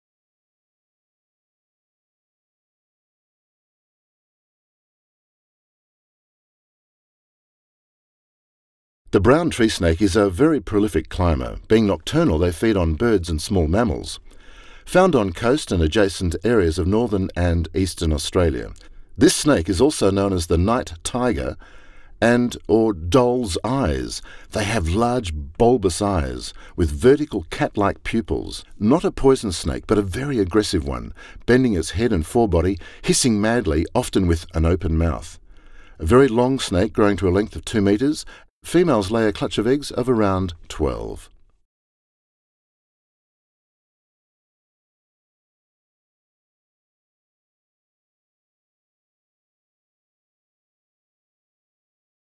BROWN TREE SNAKE
This snake found in Australia. Has small fangs at the back of it's mouth.